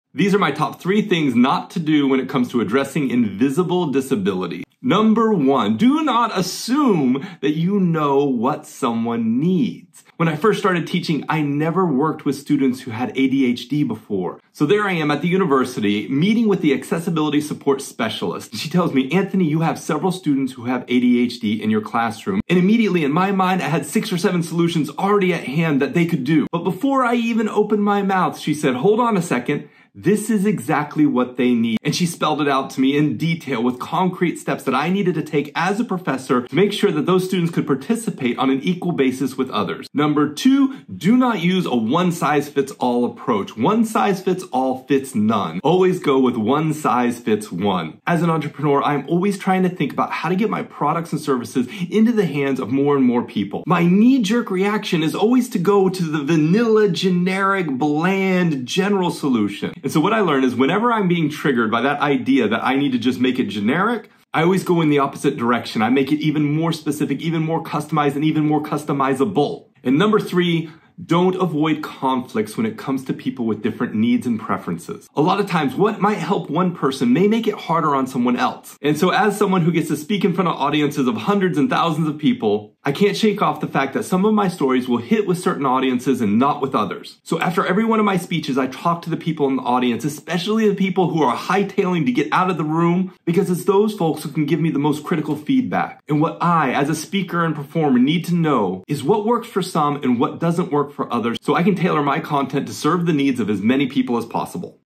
3 Things NOT to Do When Addressing Invisible Disabilities | Dr. Anthony Giannoumis
In this video, I share my top three tips on what not to do when it comes to addressing invisible disabilities. From my early teaching experiences to my entrepreneurial journey, I’ve learned valuable lessons about listening, customizing, and valuing feedback. Watch to learn more about creating inclusive environments! 🌟

Timestamps:
0:00 Introduction
0:45 Don’t Assume You Know Best
2:15 Avoid One-Size-Fits-All Approaches
3:40 Value Critical Feedback
5:10 Conclusion